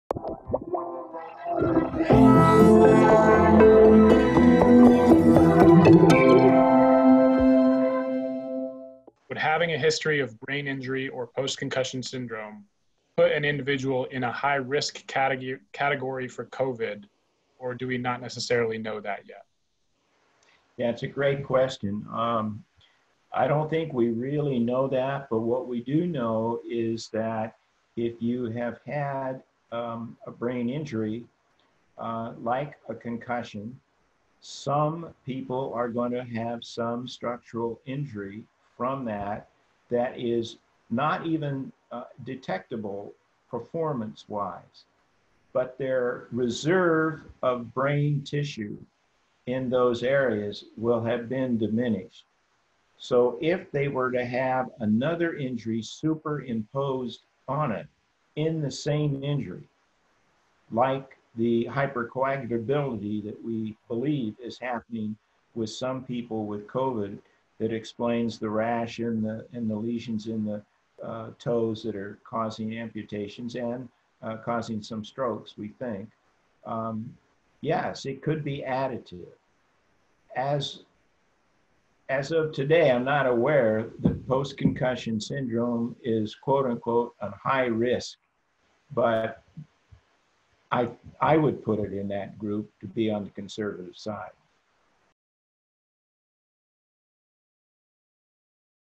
Brain injury & COVID-19 risk: Dr. Robert Cantu
CLF co-founder and medical director Dr. Robert Cantu discusses how brain injury can impact your risk for COVID-19.

Can we help you as you battle your symptoms? Submit a HelpLine Request

0:28 - Can having a brain injury put you at a higher risk for COVID?